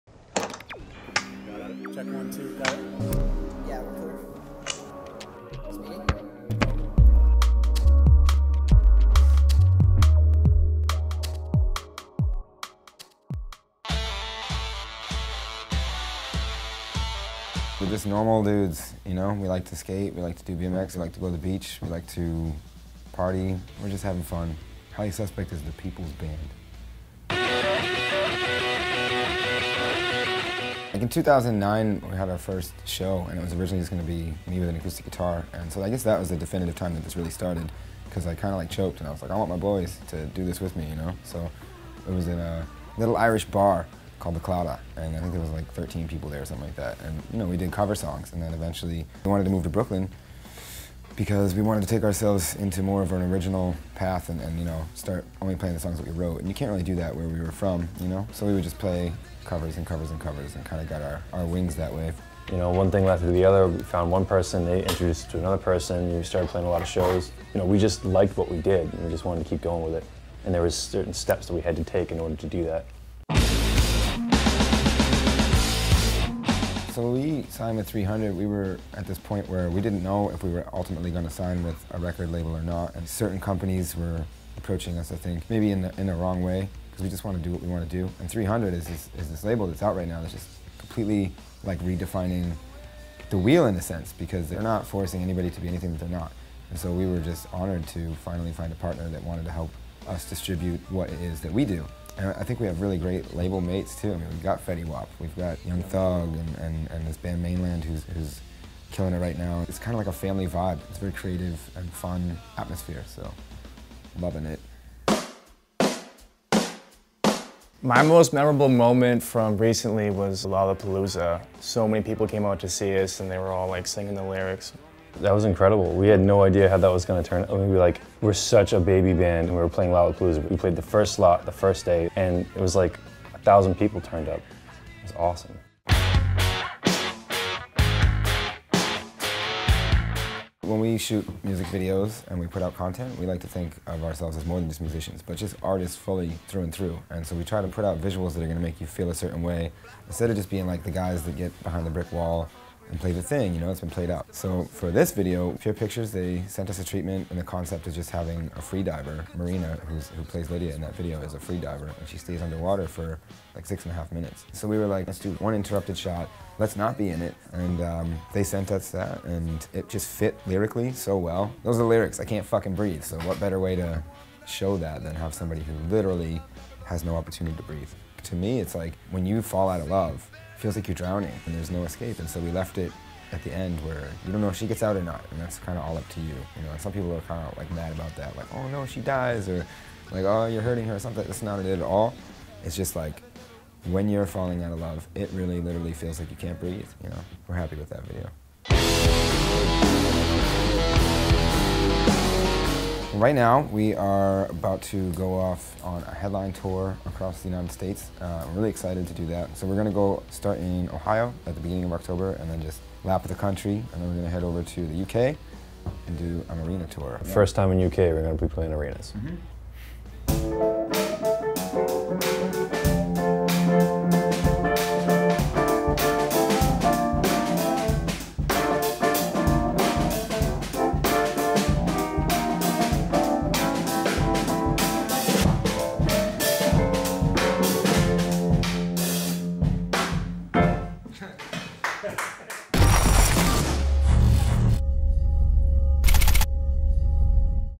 Highly Suspect talks starting the band, playing lollapalooza and signing to 300 and more
Havoc TV sat down with Highly Suspect to chat about how they started the band and more. To see more about the band make sure you